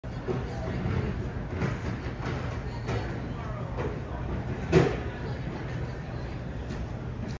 Phoenix Sky Train arrives at Terminal 4
I was planning to be in Arizona for a couple of days and took the opportunity at the Phoenix Sky Harbor International Airport and managed to get my first train being the Phoenix Sky Train, one in each direction, near the Terminal 4 station working their ways to and from both the Rental Car Center and the 44th Street/ Washington Valley Metro Rail station before heading inward.